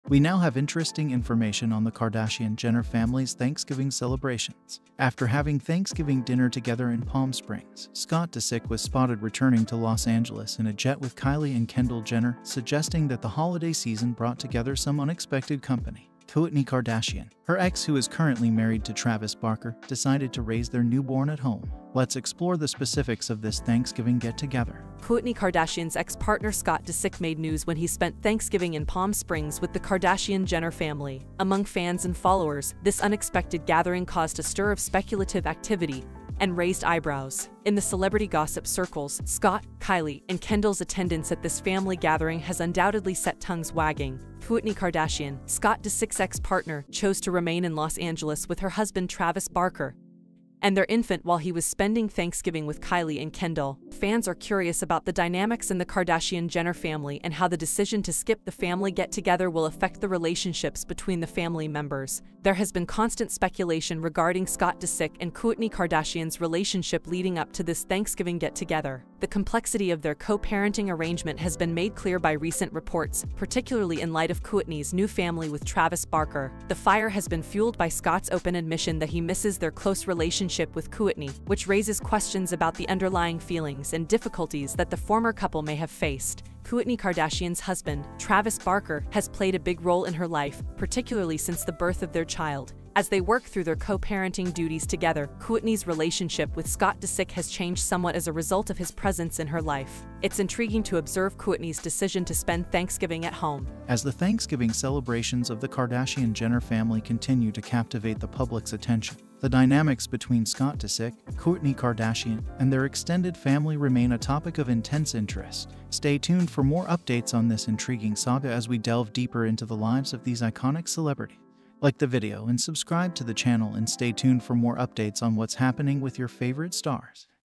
Scott Disick Spotted Jetting With Kylie & Kendall After Thanksgiving
Scott Disick, Kylie, and Kendall Jenner spending Thanksgiving together in Palm Springs, while Kourtney Kardashian stayed home with husband Travis Barker and their newborn.

Tabletop Fountain Decoration